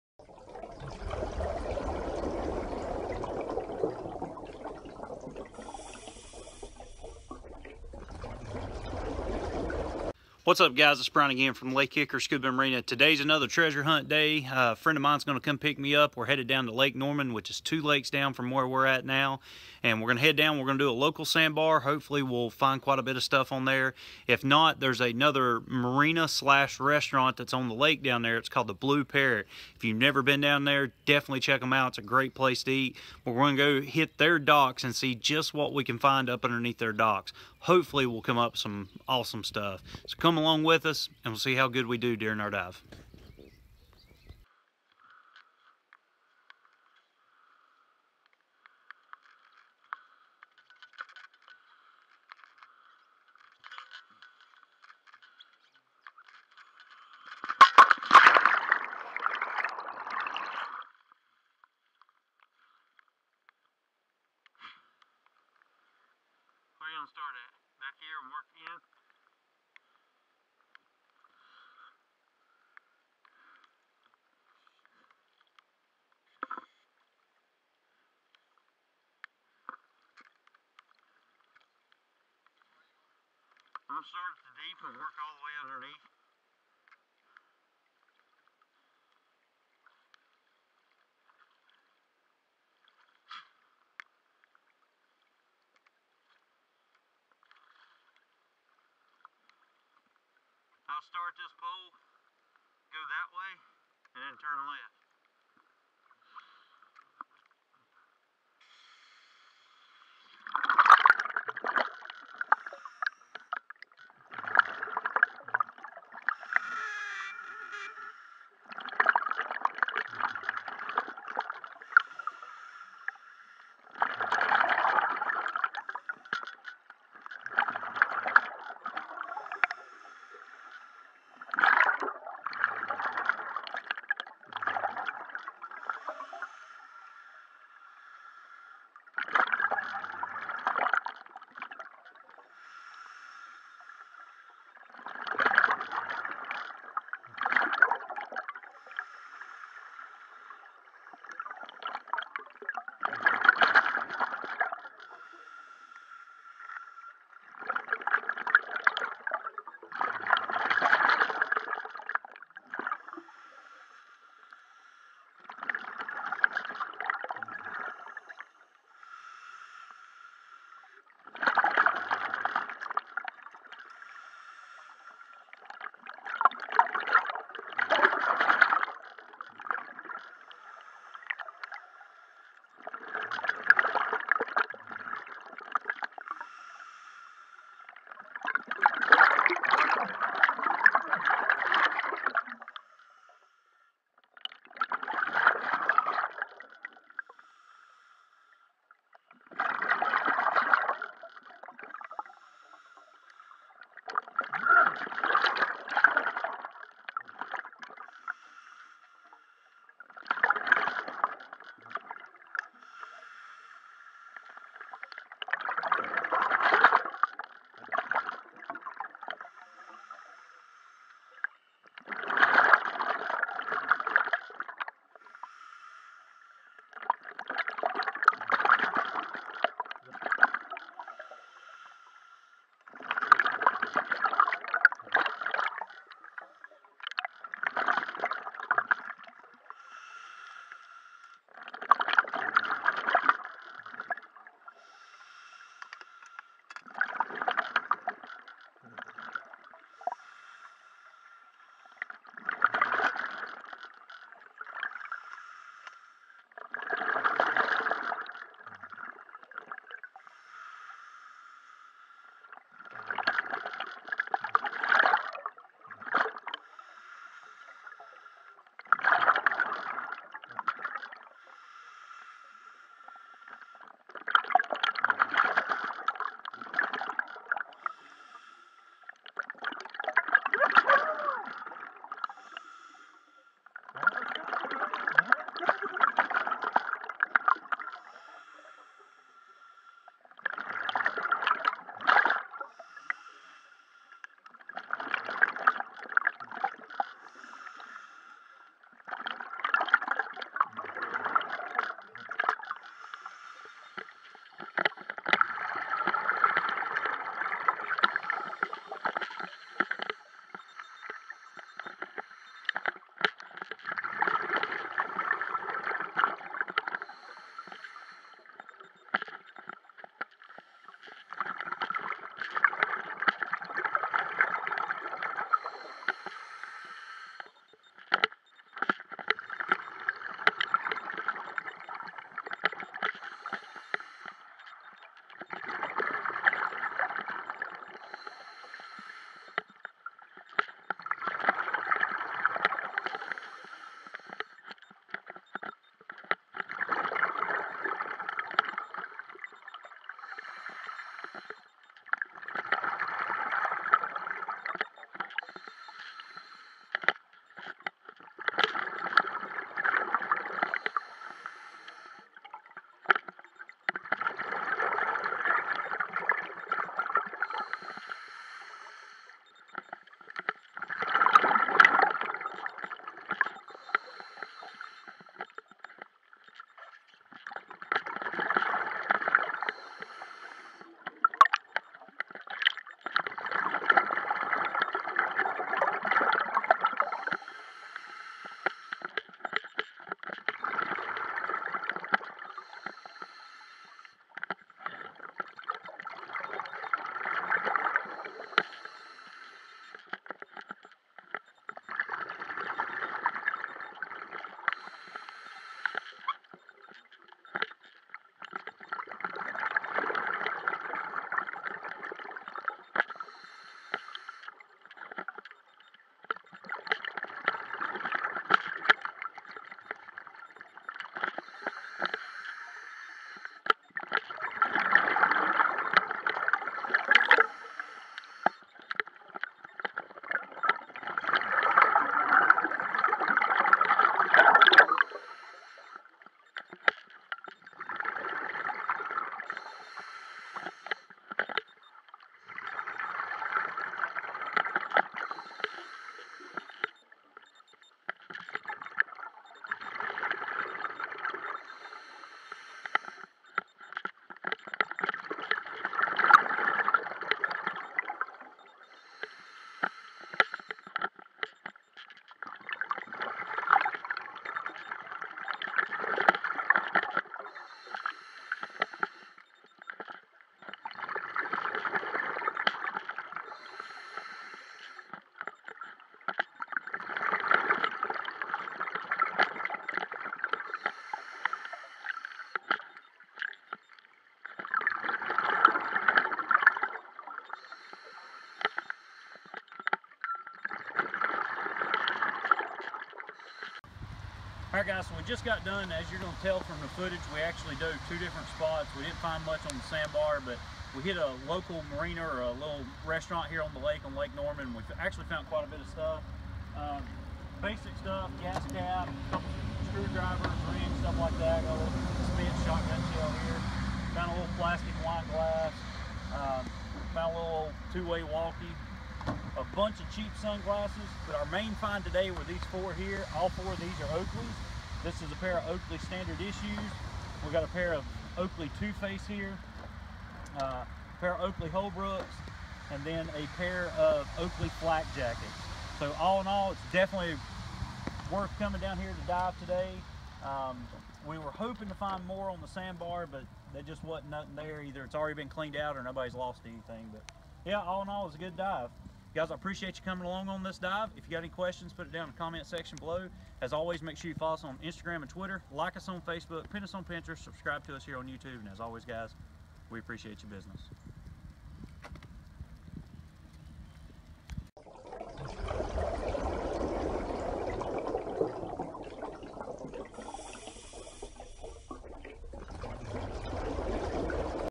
Blue Parrot Treasure Hunt Lake Norman
Decided to take the day off from work and go Under Water Treasure Hunting.  Good thing I did, we found over $600 in Oakley Sunglasses

Equipment used.
BCD - Mares Hybrid Pure
Regulator - Mares MV 12 S and Octo
Computer - Mares Matrix and Puck Air
Suit - SeaSoft Drysuit
Light - Orcatorch D620 Cannister Light